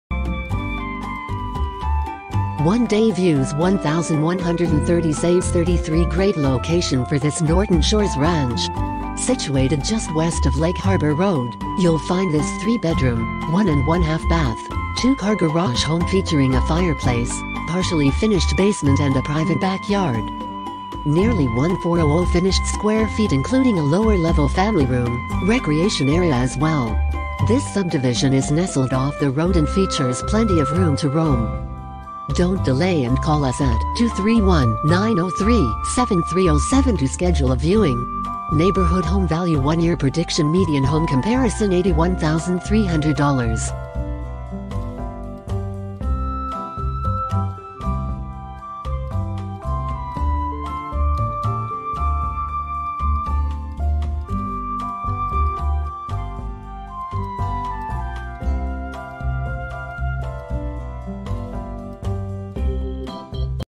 Summary - Norton Shores, , MI 49441 Home Sale
1 day Views 1,130 Saves 33 Great location for this Norton Shores ranch! Situated just West of Lake Harbor Road, you'll find this 3 bedroom, 1 1/2 bath, 2 car garage home featuring a fireplace, partially finished basement and a private backyard! Nearly 1,400 finished square feet including a lower level family room, recreation area as well! This subdivision is nestled off the road and features plenty of room to roam! Neighborhood home value One-year prediction Median home comparison $81,300

Norton Shores,   Real Estate Sale
Norton Shores,   Property Sale
Norton Shores,   Homes Sale
Norton Shores,   Single_Family Sale